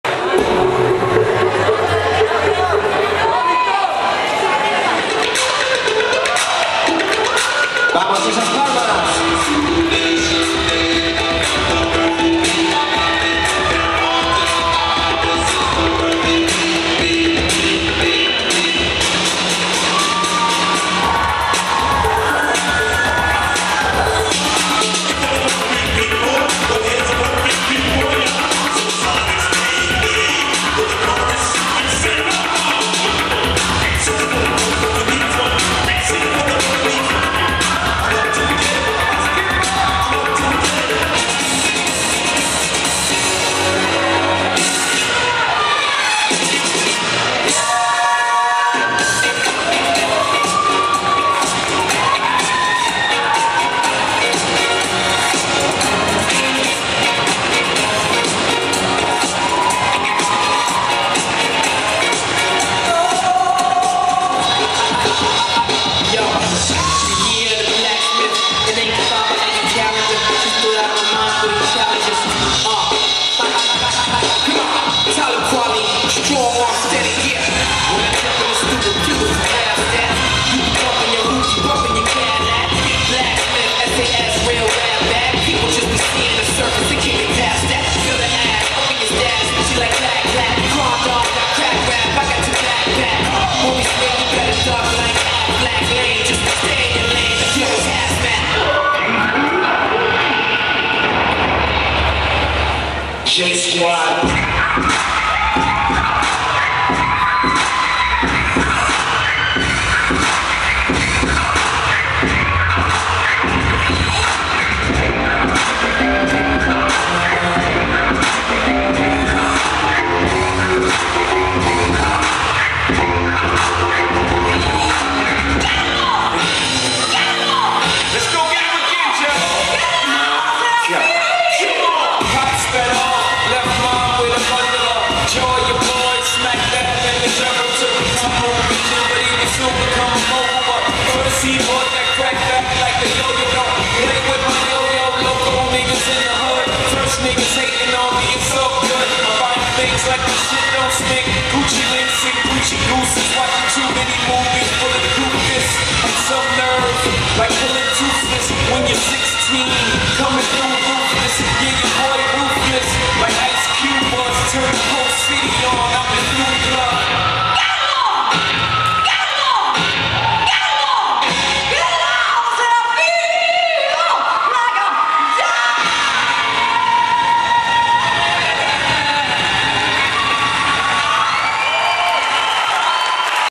Funk&break 5 Quality. segundo clasificado Absoluto
campeonato funk&break 5 celebrado en utebo el 12 y 13 de Junio Organizado por el area de juventud del Ayuntamiento de Utebo